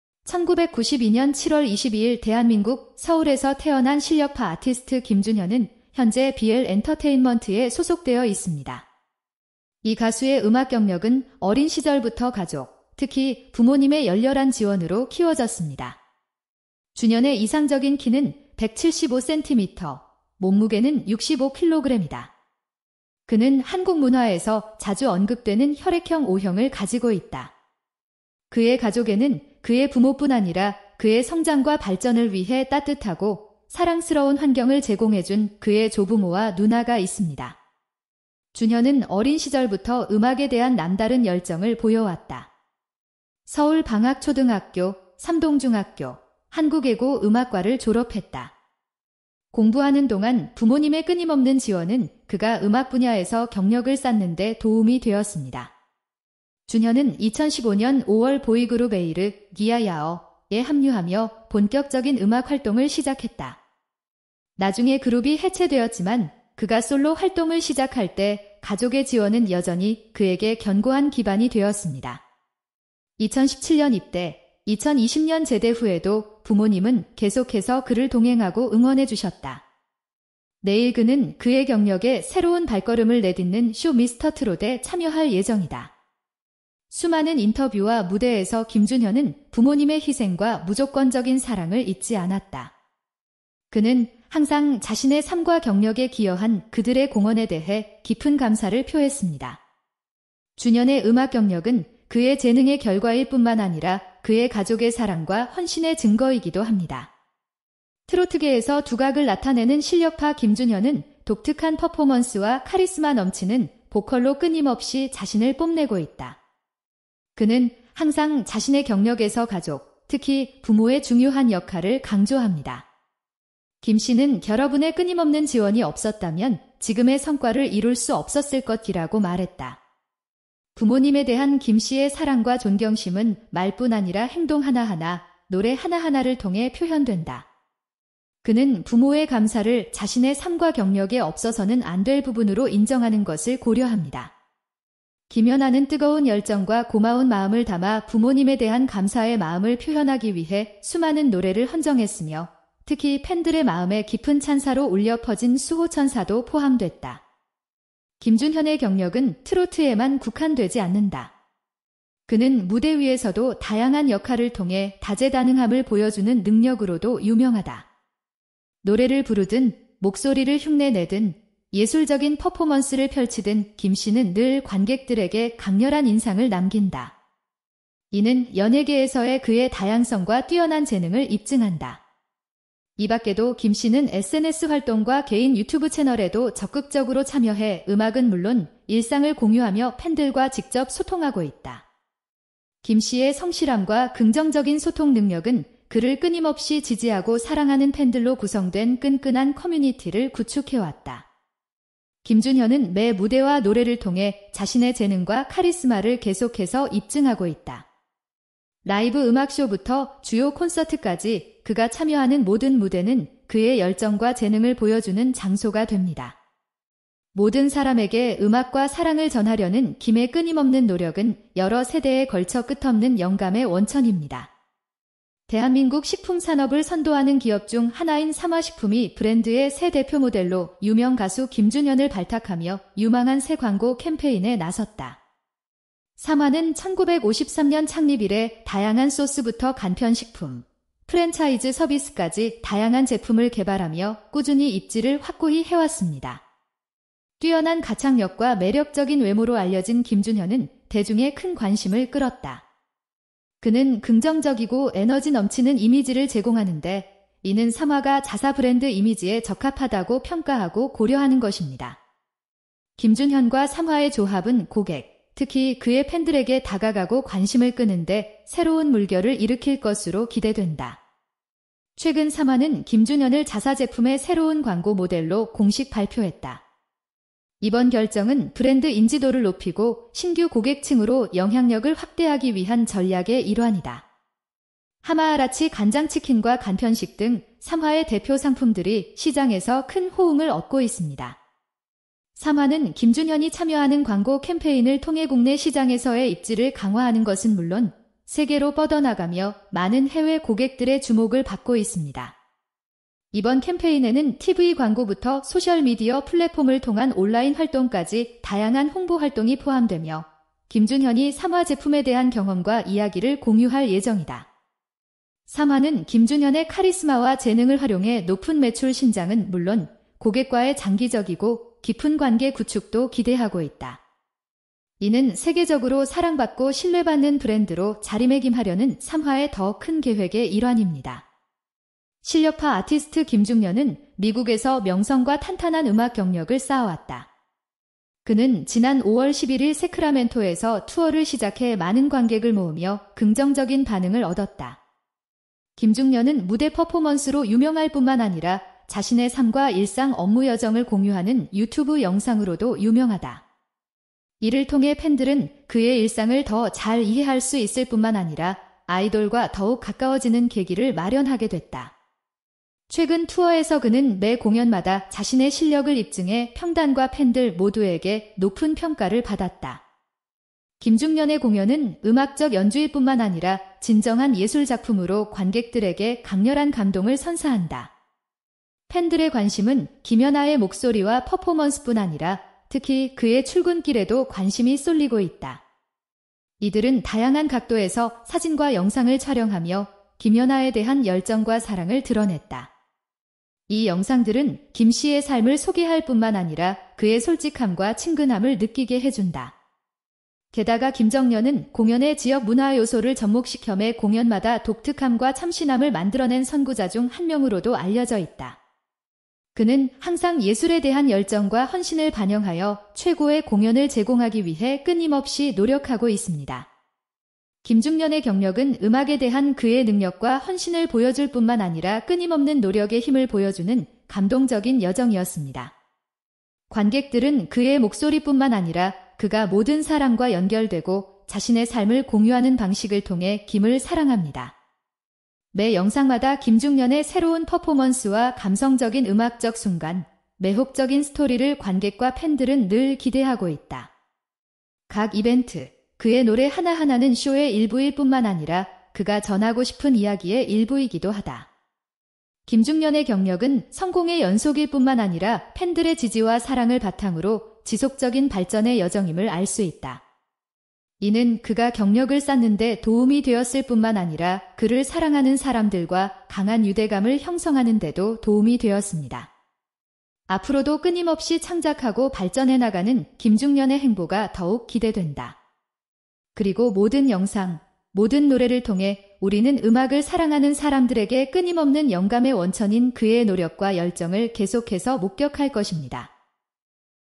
김정연이 삼화식품그룹의 브랜드 앰배서더로 정식 발탁될 것이라고는 누구도 예상하지 못했습니다! 이번 광고 계약으로 거액의 돈을 벌어들인 김정연이 온라인 커뮤니티를 떠들썩하게 했습니다
채널에 감사의 선물을 보내고, 채널 월간 멤버십에 가입하고, 영상에 좋아요를 눌러 최신 소식을 받아보세요!